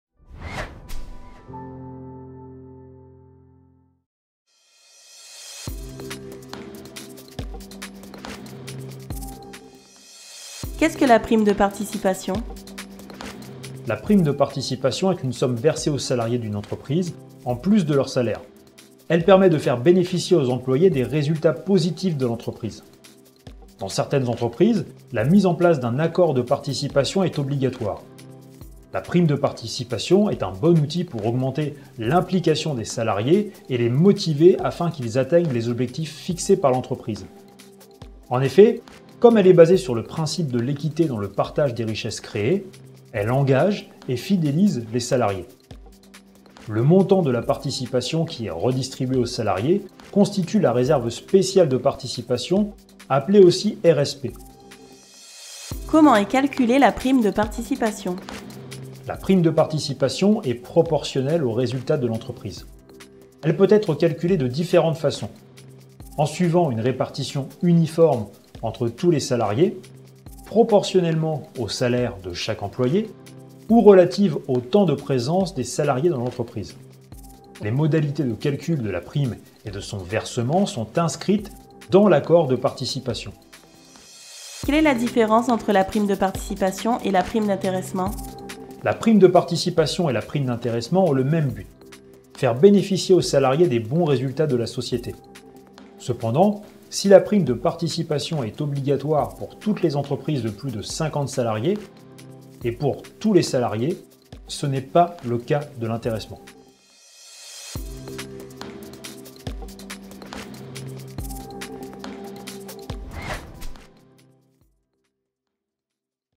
Une prime de participation, c'est quoi ? (définition, aide, lexique, tuto, explication)
Voici le sommaire de la vidéo :

Qu’est-ce que la prime de participation ?

Comment est calculée la prime de participation ?

Quelle est la différence entre la prime de participation et la prime d’intéressement ?

Sujet : Julien Dupé
Rédaction : Chloë Tadros
Présentation : Mikael Buxton
Voix off : Chloée Bernos
Design : Gaëtan Lebaron
Réalisation : Florent Lahousse
Composition sonore : Grégoire Levillain